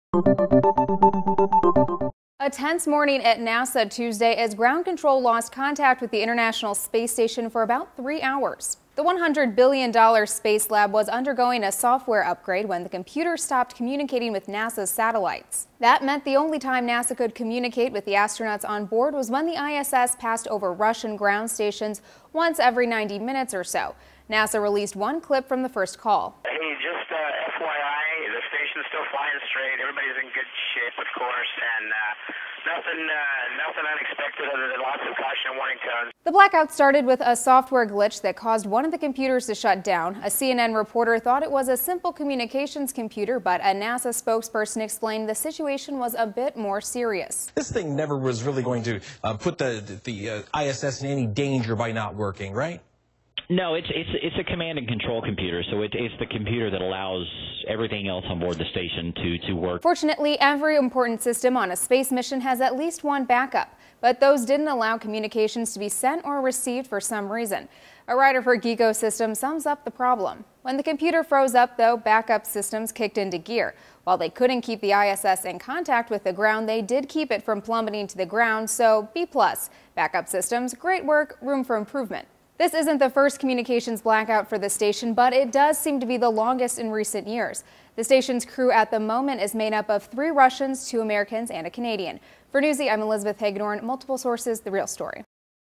NASA Loses Contact with Space Station for Three Hours
A software glitch caused a three-hour blackout during which the station could only communicate with Russian stations once every 90 minutes.